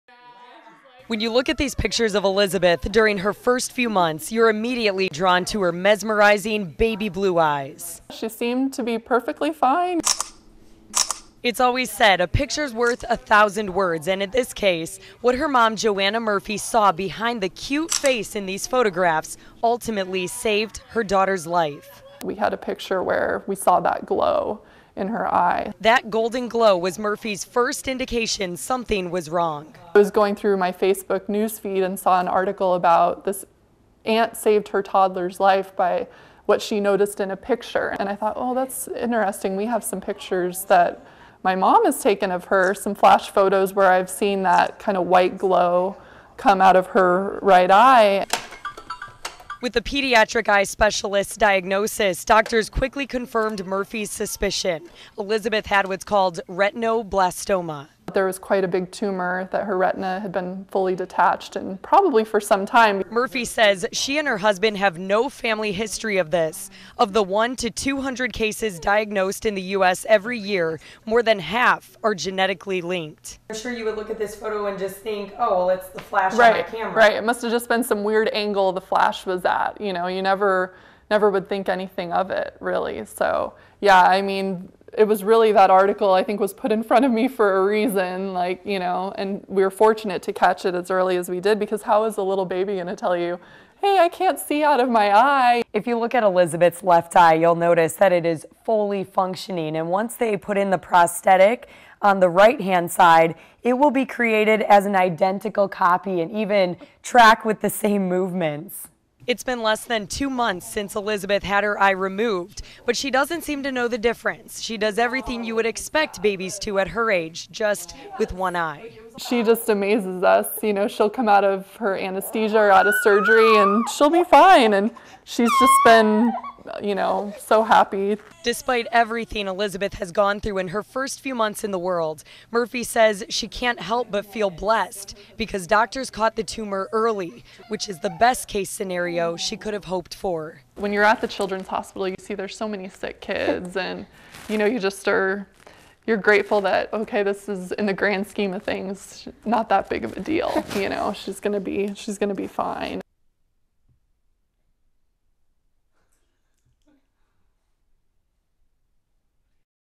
Arizona: Photo credited for saving baby's life
(KTVK/CNN) -- A golden glow in one of the infant's eyes was the first indication something was wrong.